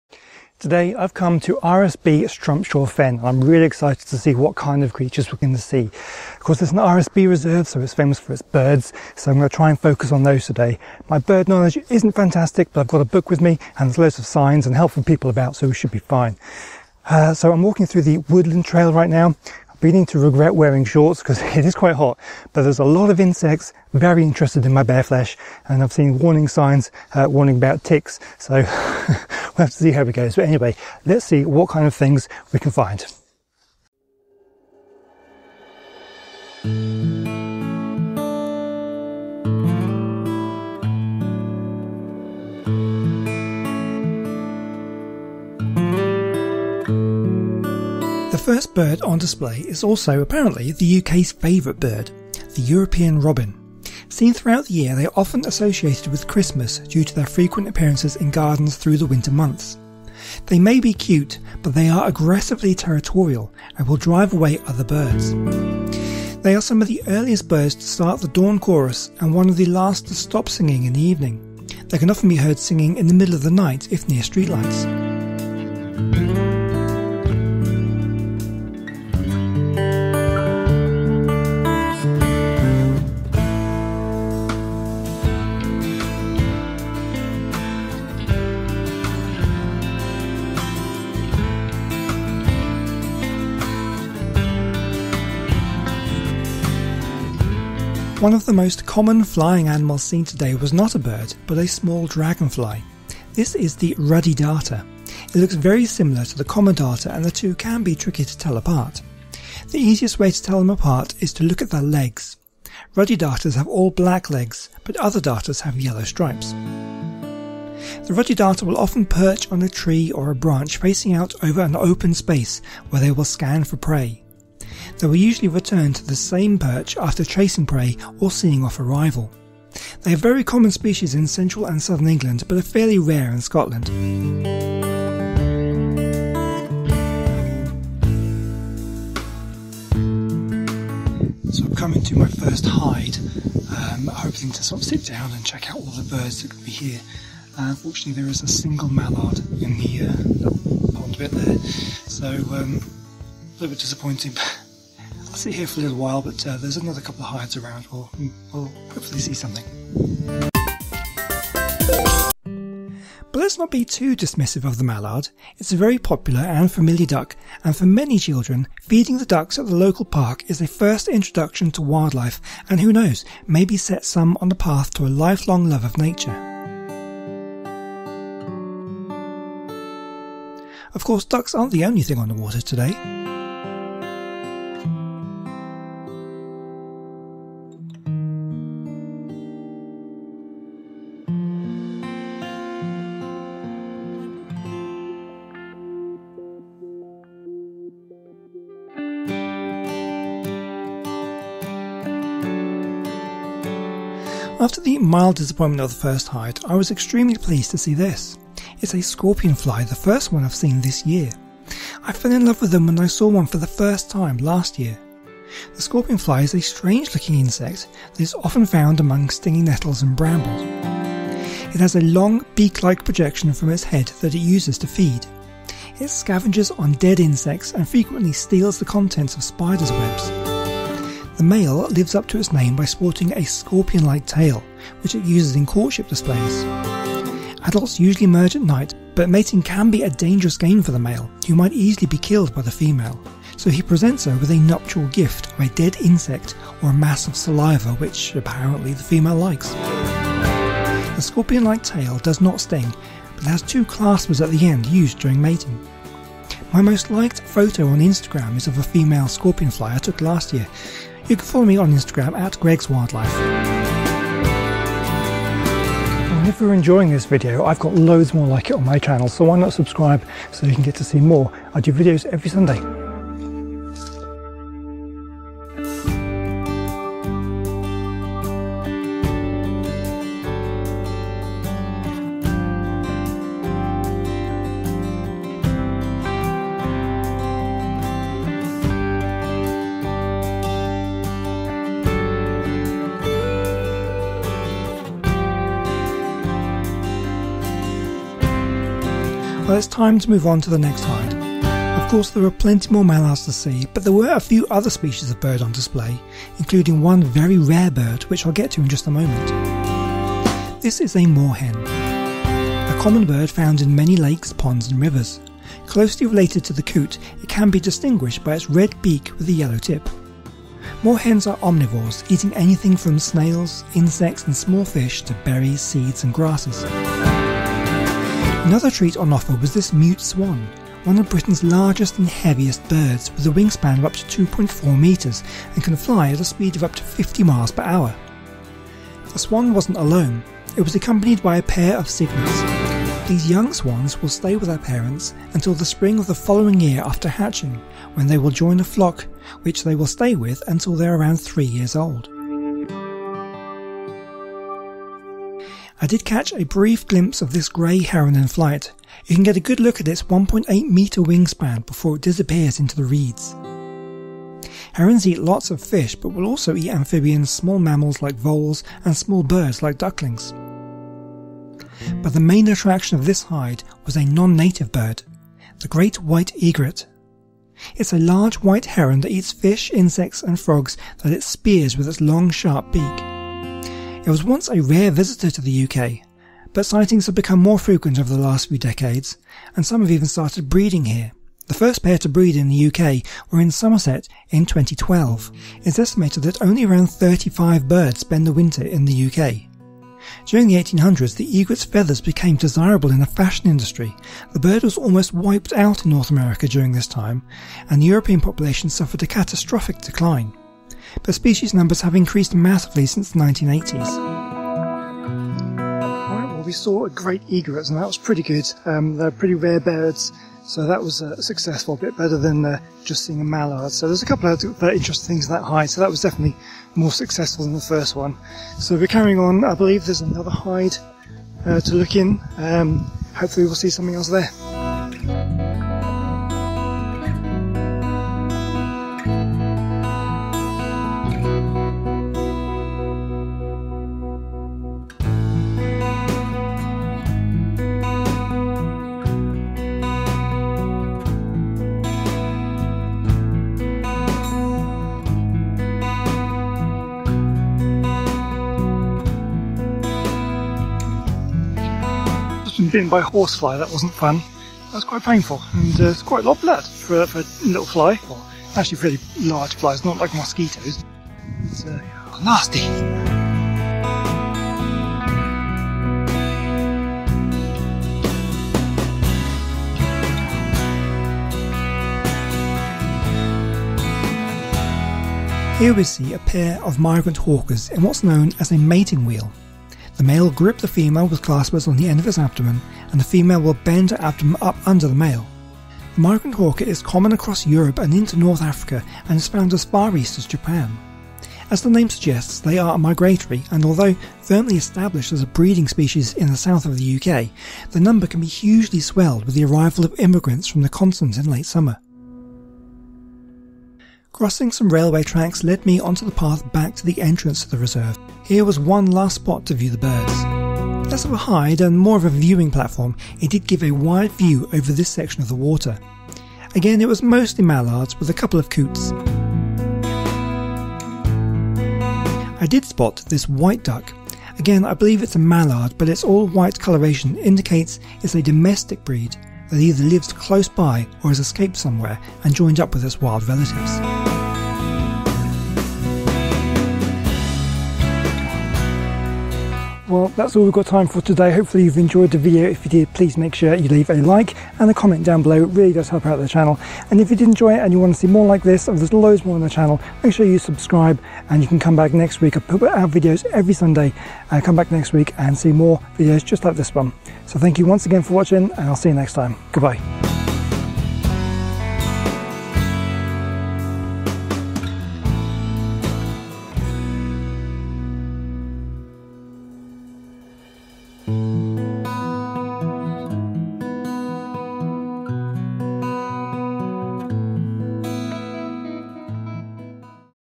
Searching For Wildlife at RSPB Strumpshaw Fen Nature Reserve
Strumpshaw Fen has a wide range of habitats and is full of rare and interesting species. Today I talk a walk around the reserve and see what I can find.
ߜ ߜ ߜ ߜ ߜ ߜ ߜ ߜ ߜ ߜ ߜ ߜ ߜ ߜ ߜ ߜ ߜ ߜ ߜ ߜ ߜ ߜ